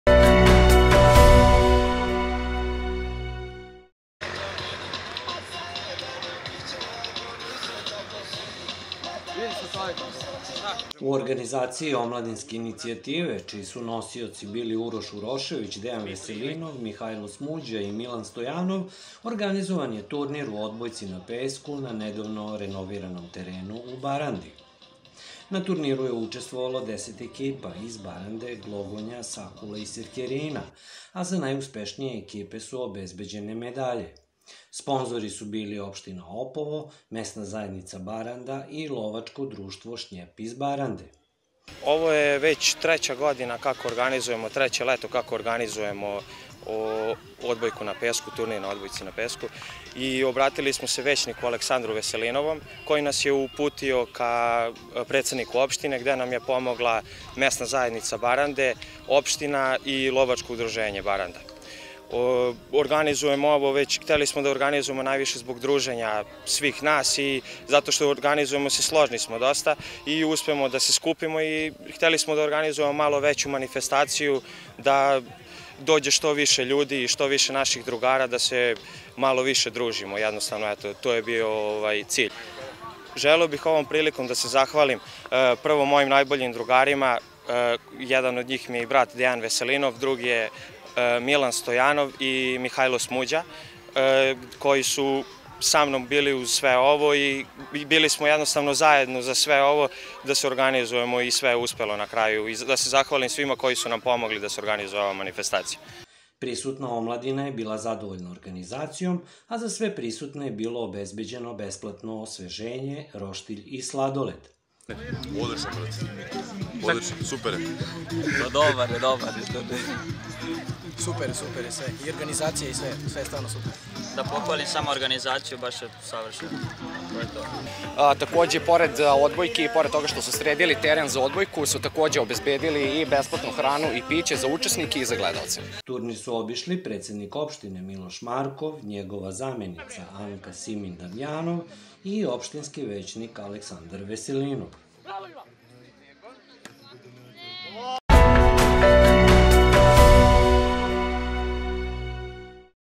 TURNIR U ODBOJCI NA PESKU BARANDA 2021
BARANDA,  5. septembar 2021 – U organizaciji omladinske inicijative čiji su nosioci bili Uroš Urošević, Dejan Veselinov, Mihajlo Smuđa i Milan Stojanov organizovan je turnir u odbojci na pesku na nedavno renoviranom terenu u Barandi.
Na turniru je učestvoavalo 10 ekipa iz Barande, Glogonja, Sakula i Sefkerina, a za najuspešnije ekipe su obezbeđene medalje. Sponzori su bili Opština Opovo, Mesna zajednica Baranda i Lovačko društvo „Šnjep“ iz Barande.